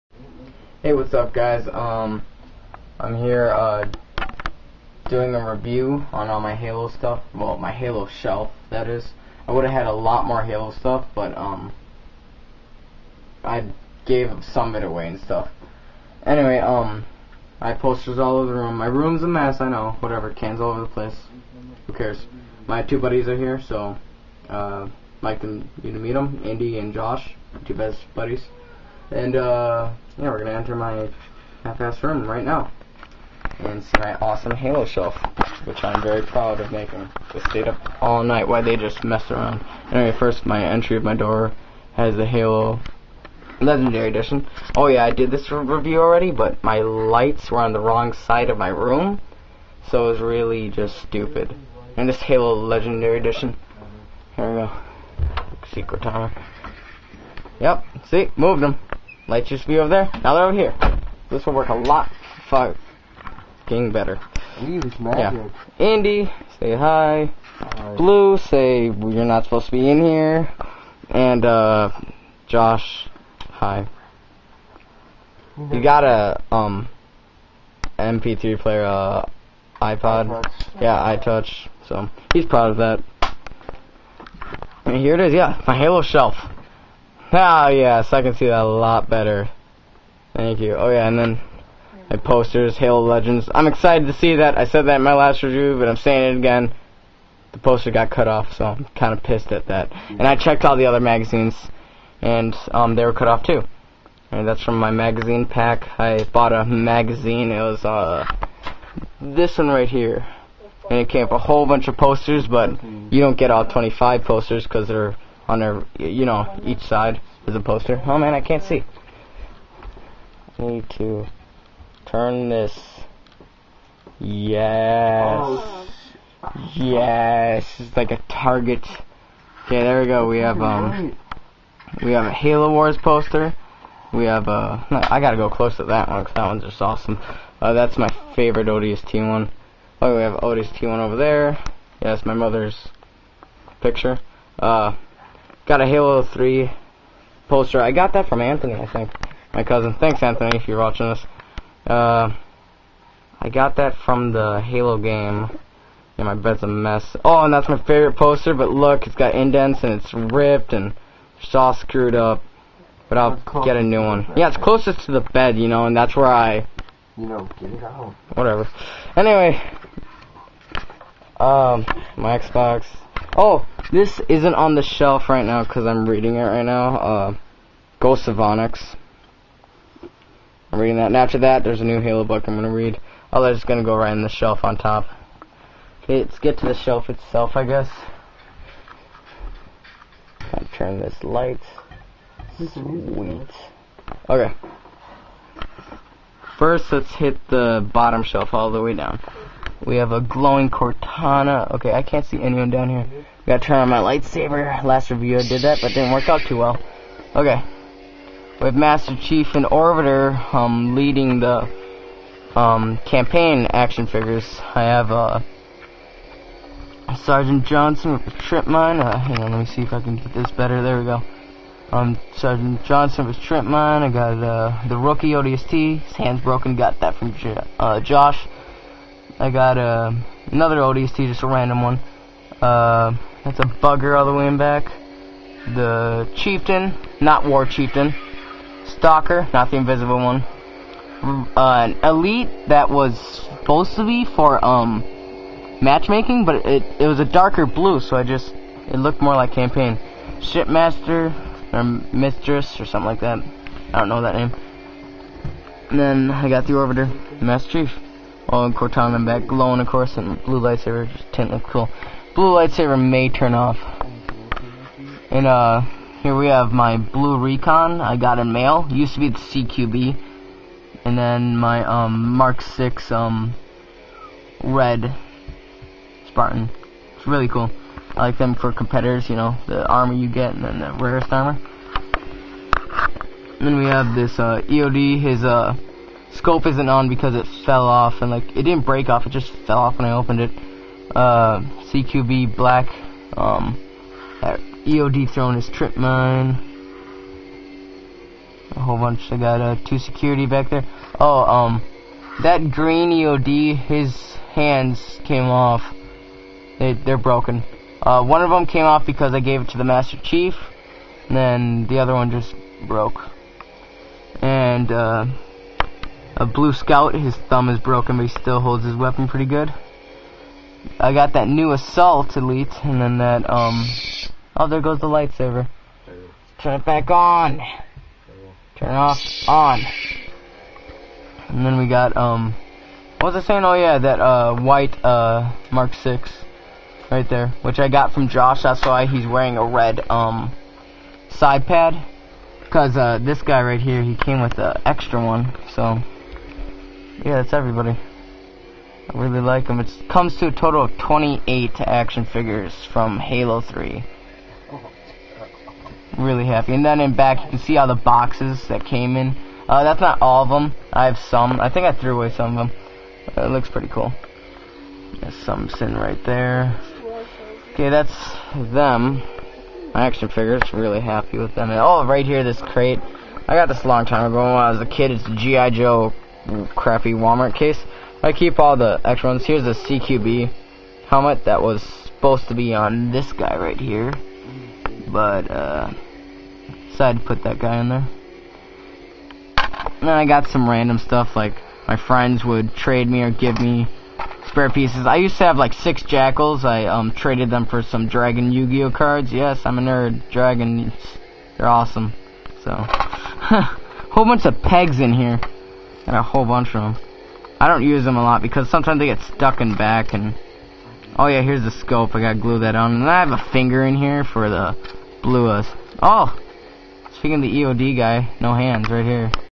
my halo shelf review part 1
this is my review on my halo shelf and posters around my messy room. lol